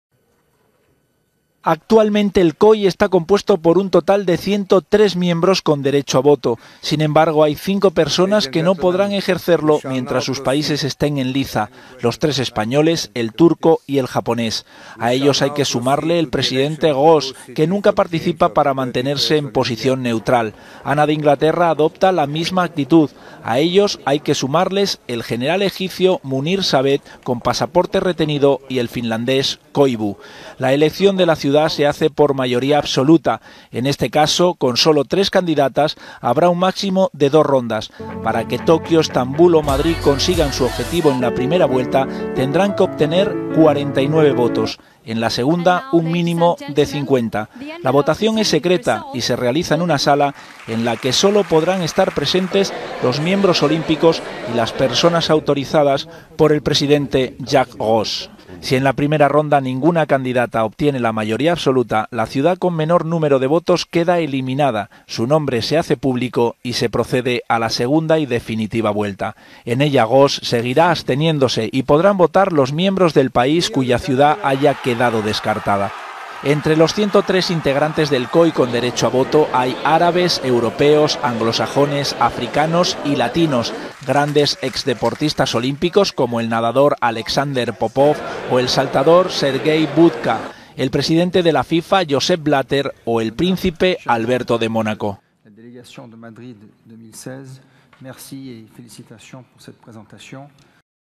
El triunfo de Madrid 2020 depende de 49 votos
La asamblea del Comité Olímpico Internacional decidirá la sede de los Juegos 2020 por medio de 97 personas. La mayoría absoluta se obtiene con 49 votos. El presidente del COI nunca vota como muestra de neutralidad.